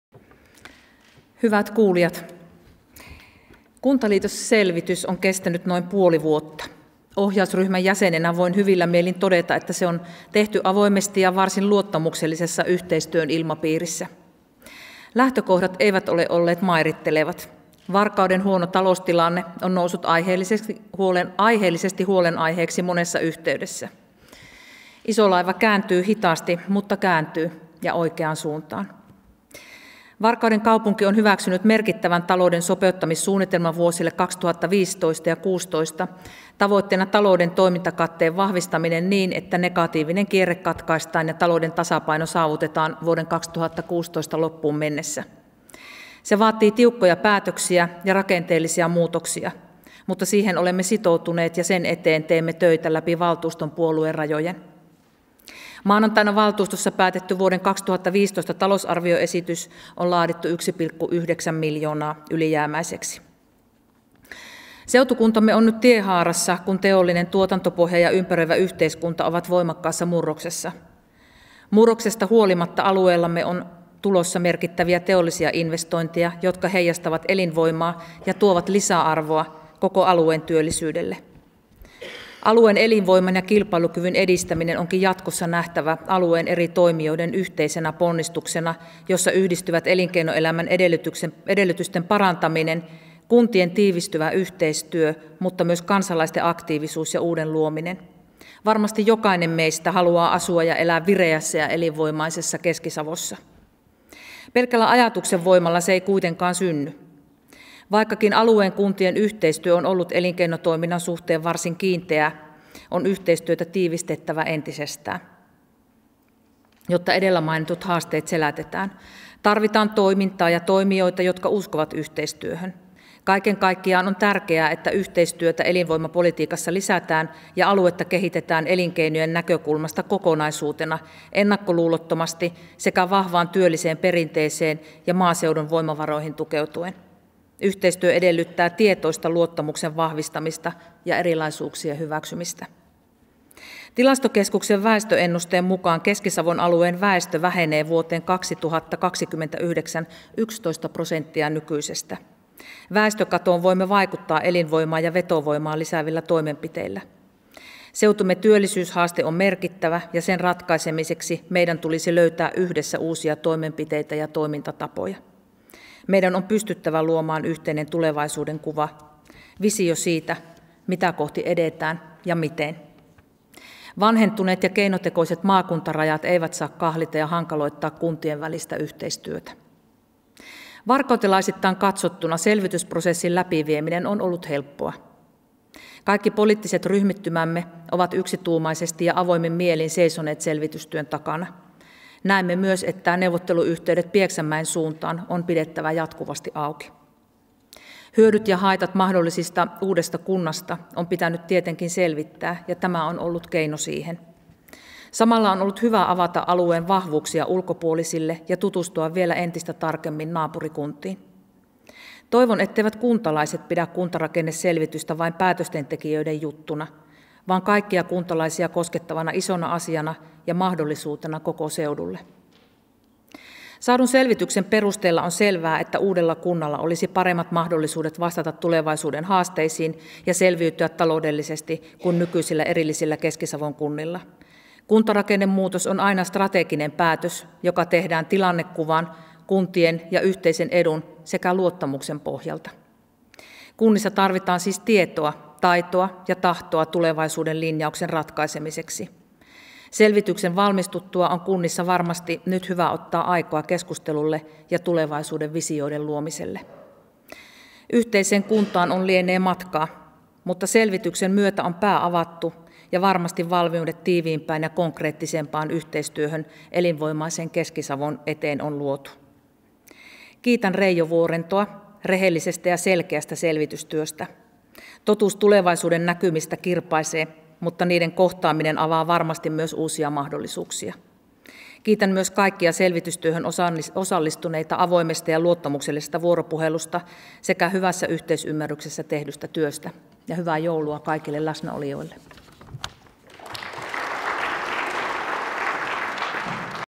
Kuntarakenneselvityksen julkistamistilaisuus 17.12.2014 - Minna Selkomaan kommenttipuheenvuoro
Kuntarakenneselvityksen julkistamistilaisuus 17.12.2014
Varkauden kaupunginhallituksen puheenjohtajan Minna Selkomaan kommenttipuheenvuoro